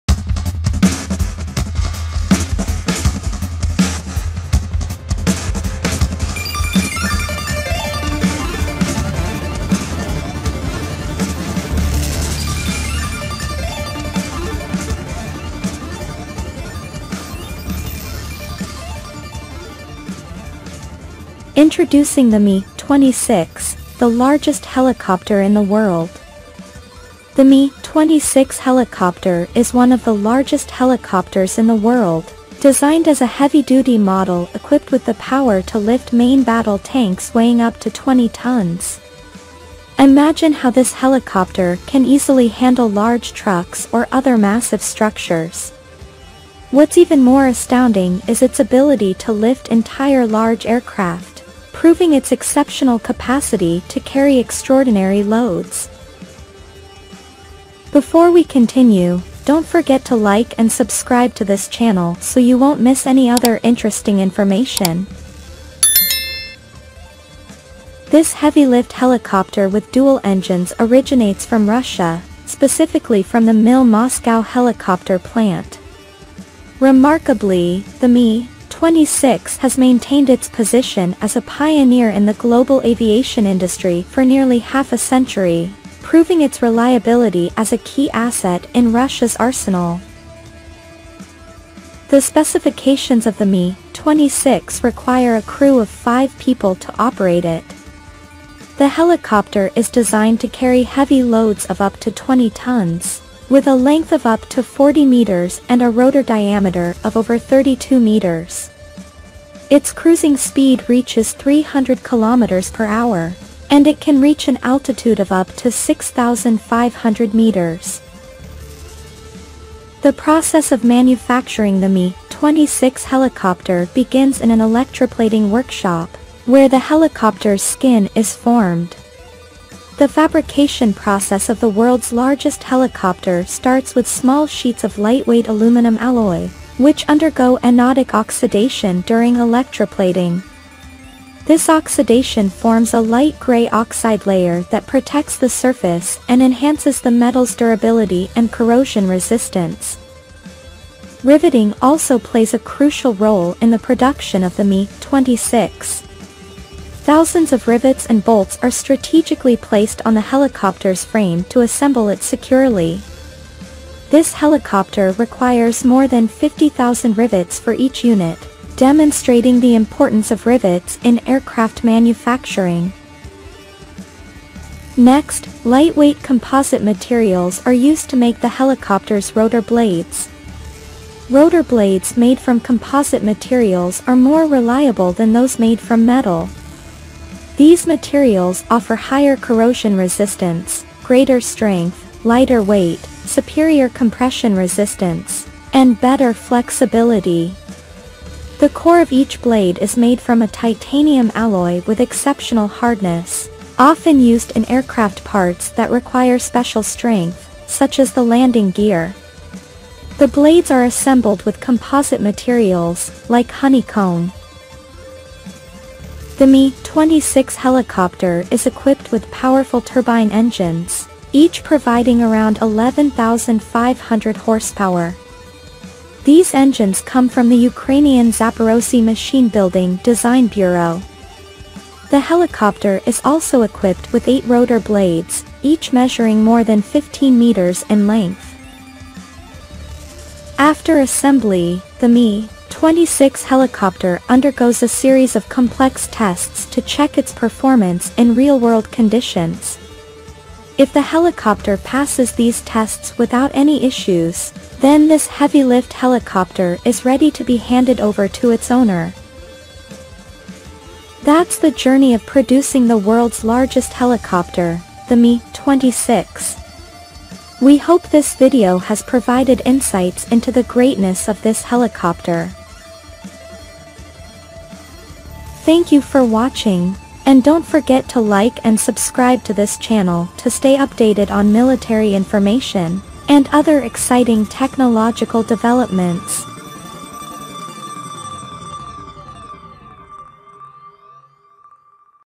Capable of Lifting Battle Planes and Tanks, This is the Largest Helicopter in the World
In this captivating YouTube video, we take you on an incredible journey to discover the world's largest helicopter, a true marvel capable of effortlessly lifting battle planes and tanks. This awe-inspiring aerial vehicle stands as a testament to modern engineering's ingenuity, pushing the boundaries of what's possible in aviation. From its impressive size to its remarkable lifting power, this helicopter will leave every viewer spellbound.

Throughout the video, we delve into the history behind this astounding helicopter, exploring its cutting-edge technology and versatile applications. Witness breathtaking aerial footage showcasing the might and agility of this giant as it lifts battle planes and tanks with ease.

This video caters to aviation enthusiasts and military technology aficionados eager to learn about advanced developments in the field. Whether you're a technology fan or simply fascinated by impressive machinery, this helicopter will undoubtedly capture your imagination.

Join us to experience the jaw-dropping wonder of this sky giant. Don't miss this opportunity to witness this remarkable helicopter in action and learn more about its incredible performance.